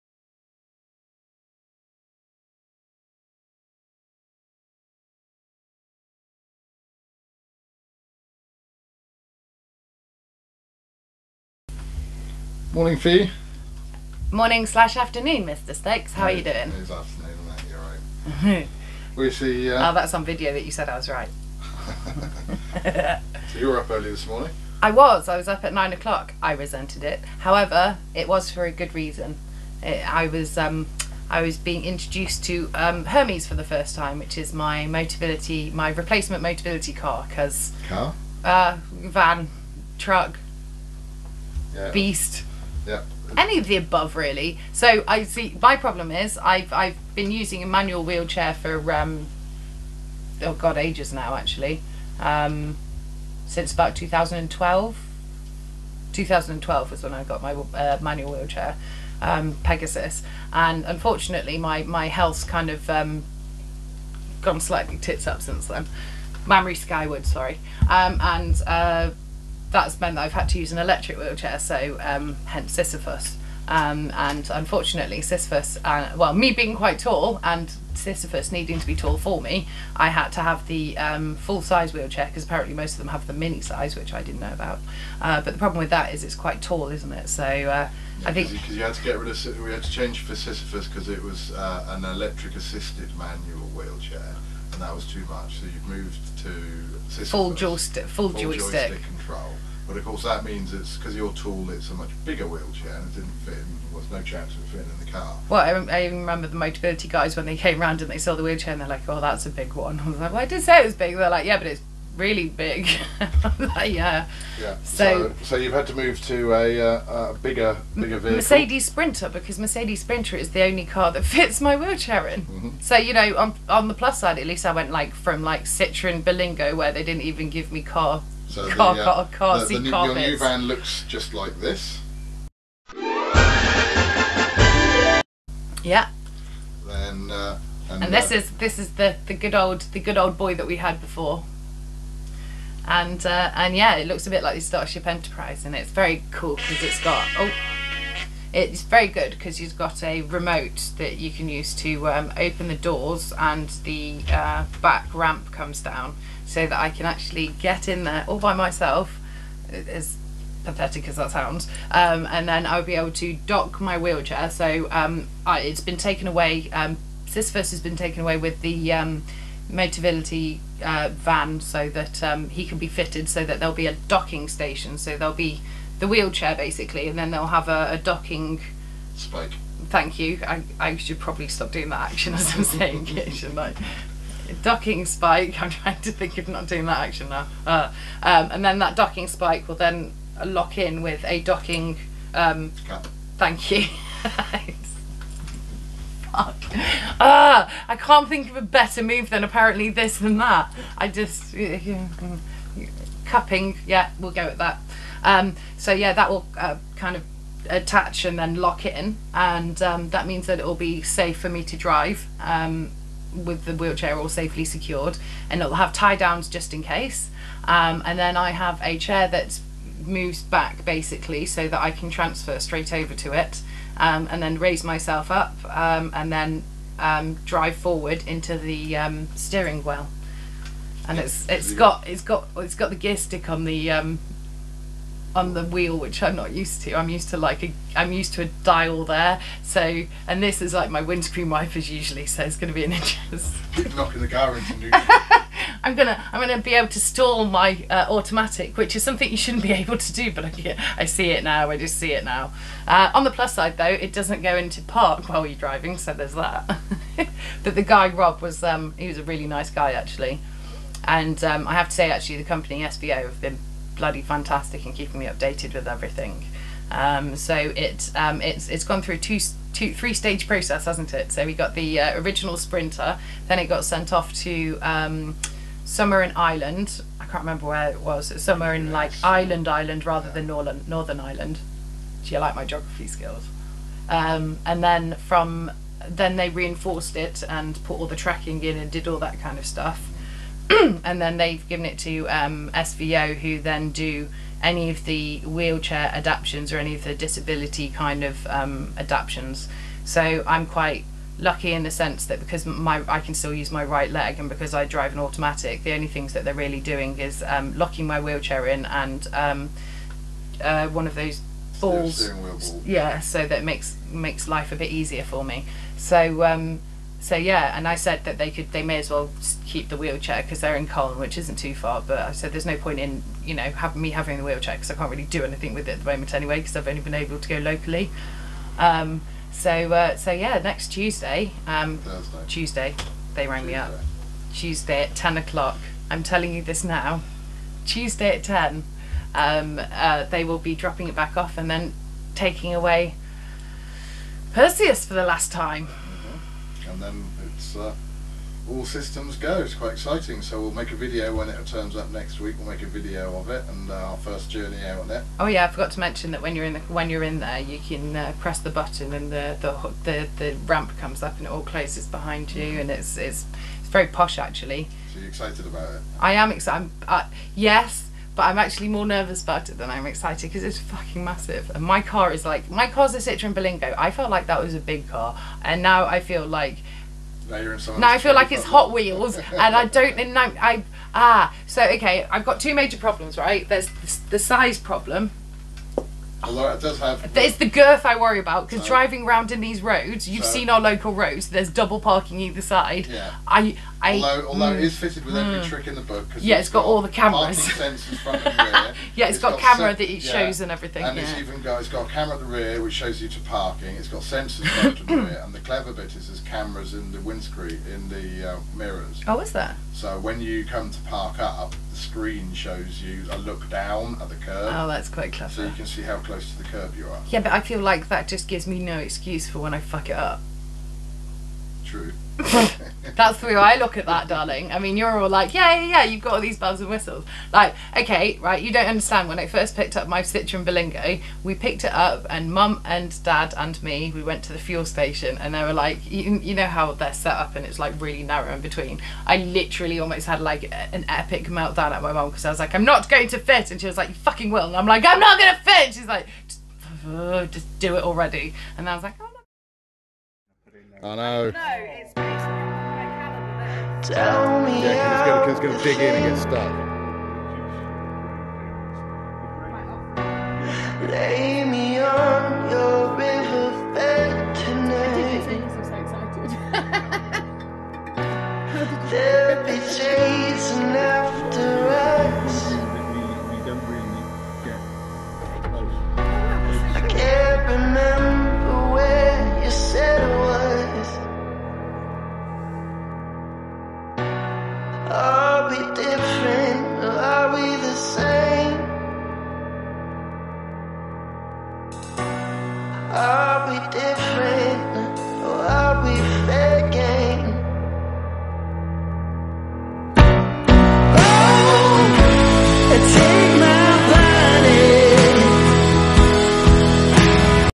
Hermes arrives for his fitting!!
Well it’s long awaited but he is finally here!! Rob, the nice gent who is fitting the wheelchair docking station and other aids to my new motability van, Hermes.

This is my introduction to him before he goes off for his final sprucing up and fittings. This time next Tuesday I shall have my new motability van. This means everything to me, I’ve had to start using a bigger fully electric wheelchair which is rather large so for over a year we’ve been limited in what we’ve been able to actually do.

So this time next Tuesday Fi be the owner of Hermes. Now she got to learn to drive a bloody bus in comparison to her car!!